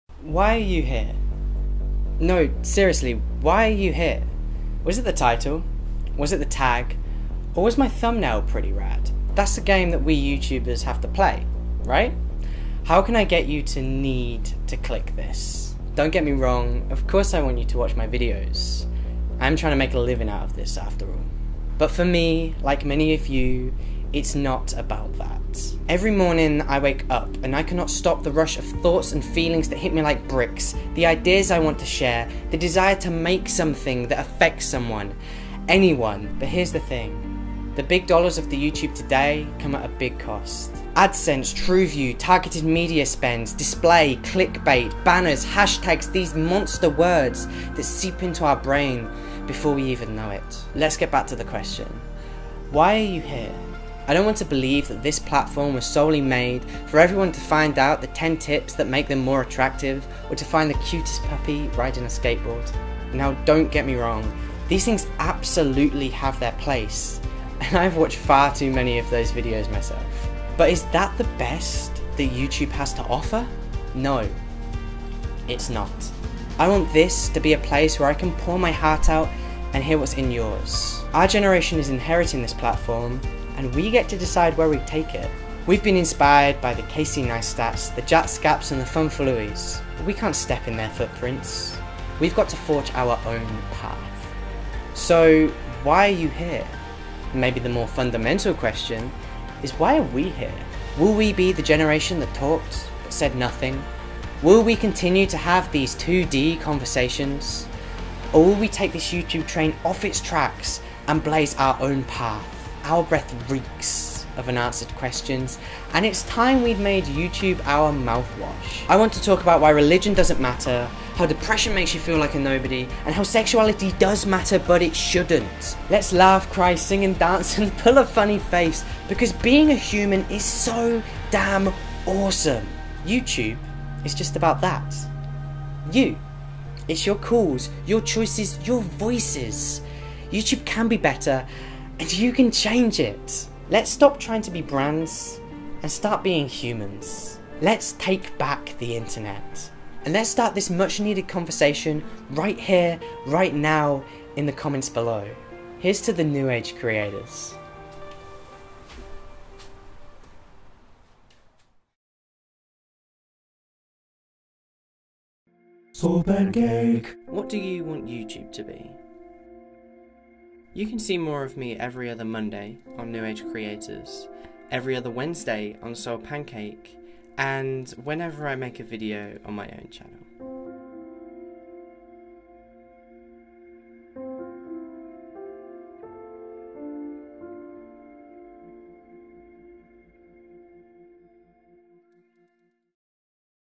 Why Are You Here? | New Age Creators
"YouTube is just about that... YOU. YouTube can be better, and YOU can change it."
Let's start putting our best foot forward on the internet and make stuff that matters! Yes, there is a time and place for the cutest puppies and the perfect selfies, but that can't be ALL we care about seeing. On YouTube, YOU can be the creator. So join us in creating things that make a difference.